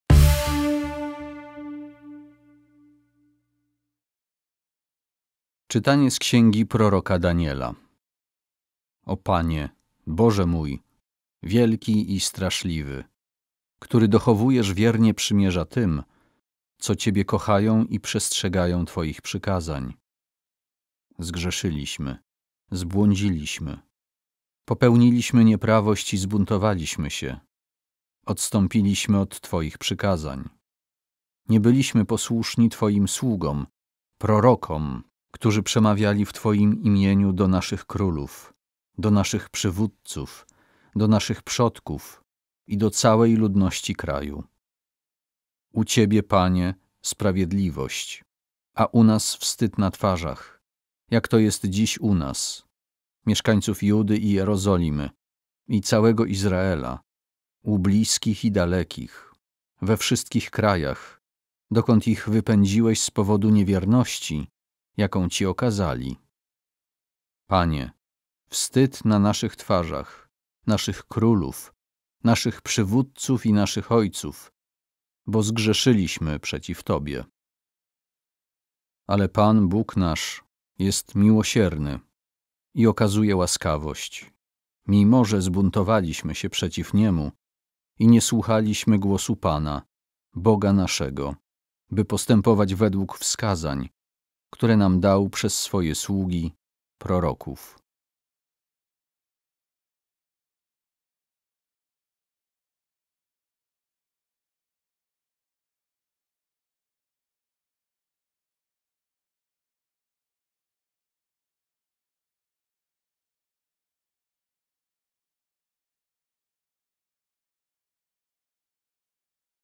PierwszeCzytanie | 1 marca 2021
1 marca 2021
Dn 9, 4b-10
Czytanie z Księgi proroka Daniela
O Panie, Boże mój, wielki i straszliwy, który dochowujesz wiernie przymierza tym, co Ciebie kochają i przestrzegają Twoich przykazań.
Zgrzeszyliśmy, zbłądziliśmy, popełniliśmy nieprawość i zbuntowaliśmy się, odstąpiliśmy od Twoich przykazań. Nie byliśmy posłuszni Twoim sługom, prorokom, którzy przemawiali w Twoim imieniu do naszych królów, do naszych przywódców, do naszych przodków i do całej ludności kraju.
U Ciebie, Panie, sprawiedliwość, a u nas wstyd na twarzach, jak to jest dziś u nas – mieszkańców Judy i Jerozolimy, i całego Izraela, u bliskich i dalekich, we wszystkich krajach, dokąd ich wypędziłeś z powodu niewierności, jaką Ci okazali.
Panie! Wstyd na naszych twarzach, naszych królów, naszych przywódców i naszych ojców, bo zgrzeszyliśmy przeciw Tobie. Ale Pan, Bóg nasz, jest miłosierny i okazuje łaskawość, mimo że zbuntowaliśmy się przeciw Niemu i nie słuchaliśmy głosu Pana, Boga naszego, by postępować według wskazań, które nam dał przez swoje sługi, proroków.